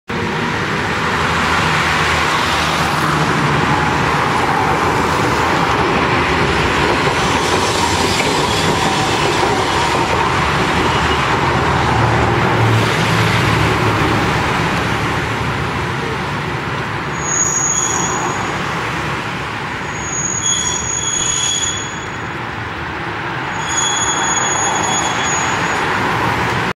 伊予鉄道モハ50形 大街道付近にて
伊予鉄道市内線大街道付近を走行するモハ50形（57）。1系統松山市内環状線下り（古町始発）松山市行。同区間の同方面行は、本町六丁目行・JR松山経由小町行・道後温泉始発松山市行も通行する。